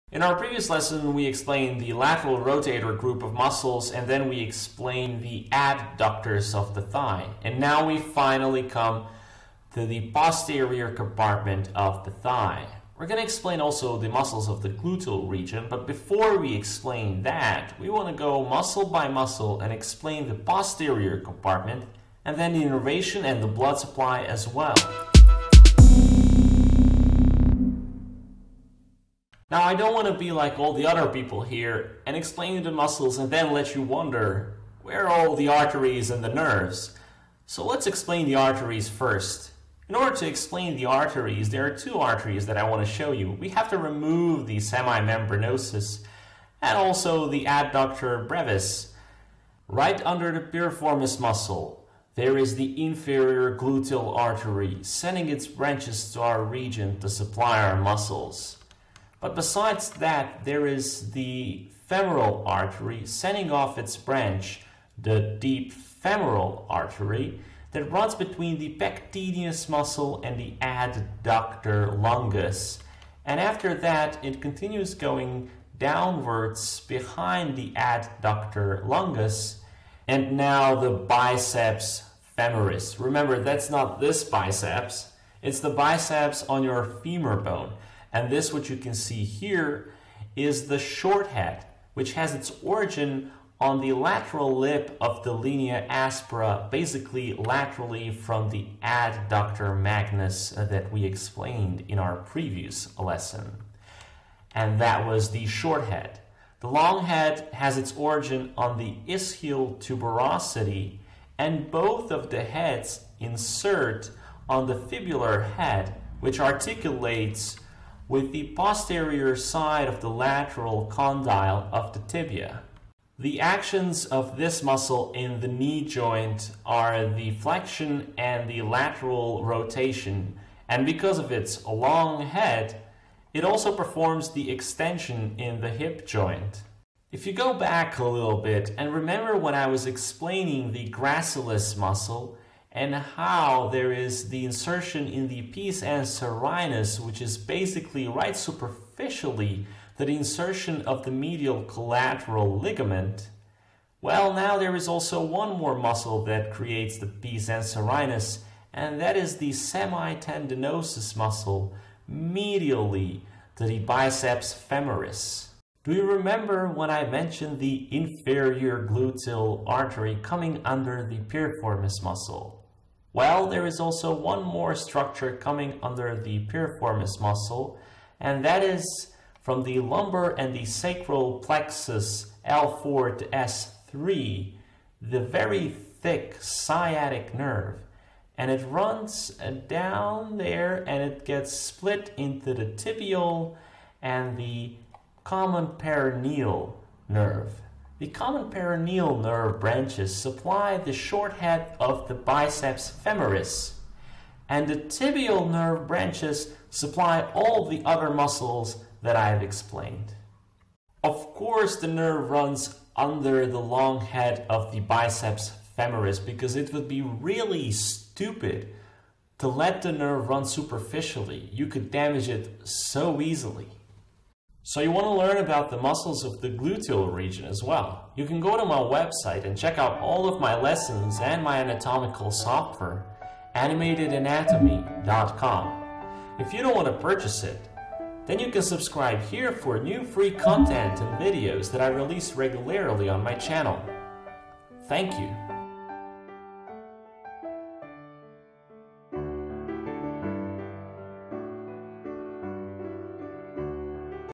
Thigh Muscles - Posterior Compartment and Sciatic Nerve
A video about the thigh muscles - the posterior compartment with the sciatic nerve and the deep femoral artery explained.

It is important to mention the semimembranosus and the semitendinosus muscles and the biceps femoris with its two heads that originate on the femur and the tuberosity of the ischium. These thigh muscels in the posterior compartment are innervated by the sciatic nerve and the deep femoral artery as well as the inferior gluetal artery.
Music.